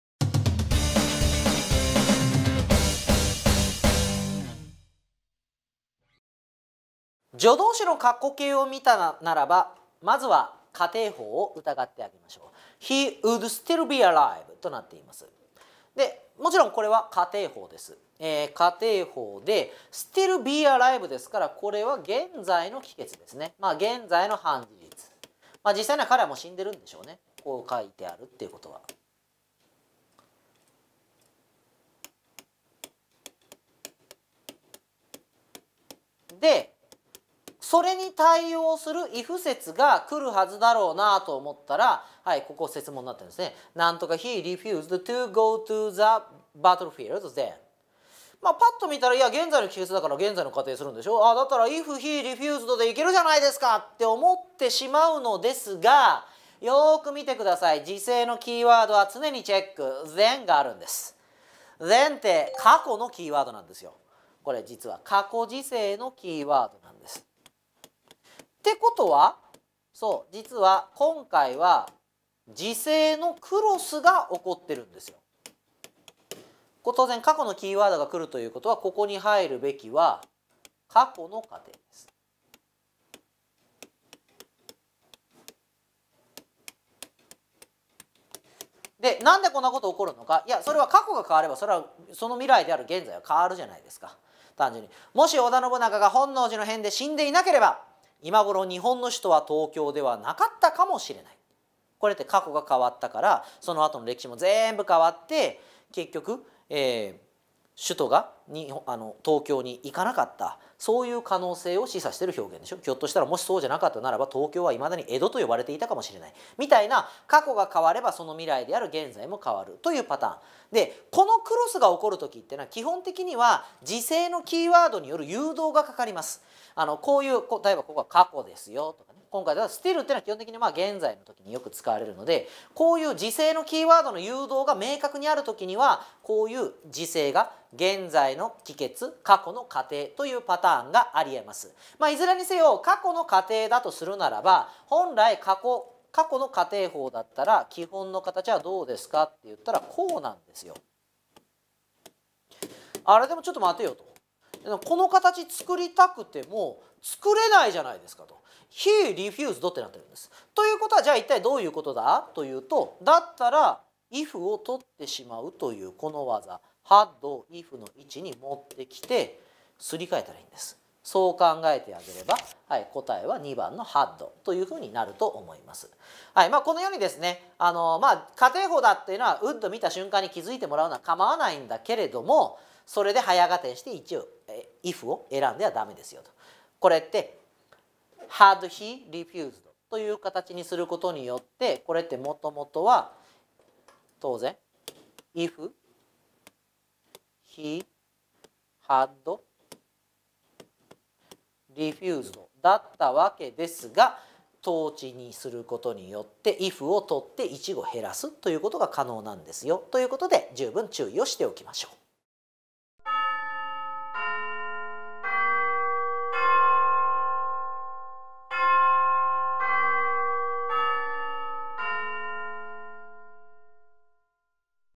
マイドリ155_条件・仮定
次の問題を解説しています。挑戦したい方は制限時間30秒でトライしてみて下さい！
適語選択
He would still be alive (     ) he refused to go to the battlefield then.
1. if　2. had　3. provided　4. unless